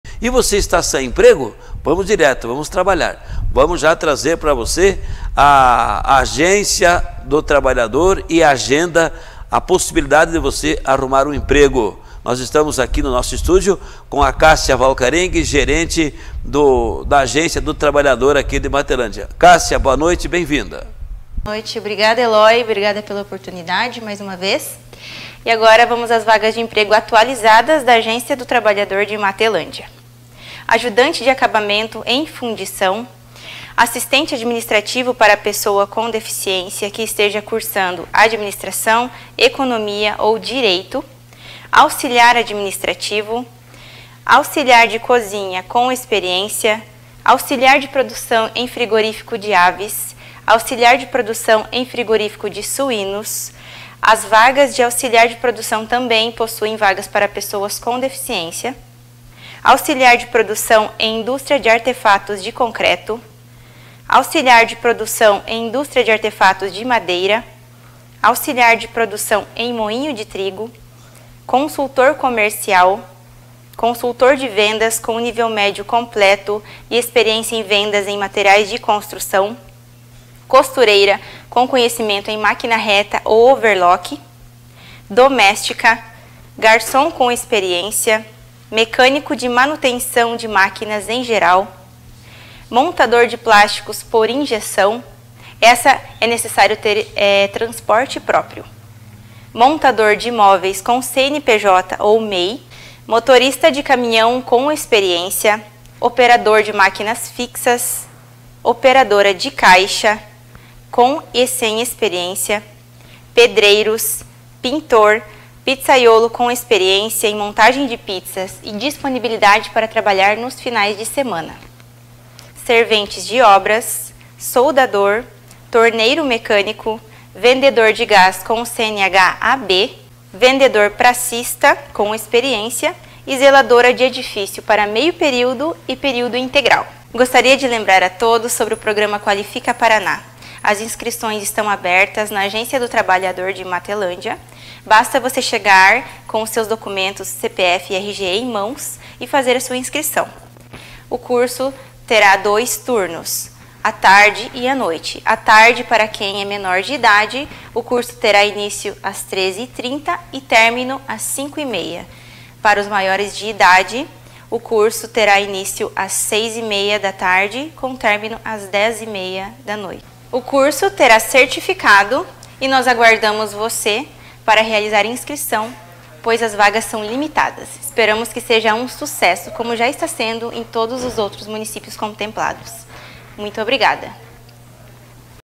Confira as vagas de emprego da Agencia do trabalhador em Matelândia com Cassia Valcarenghi
Transmissões esportivas e em geral, programas ao vivo.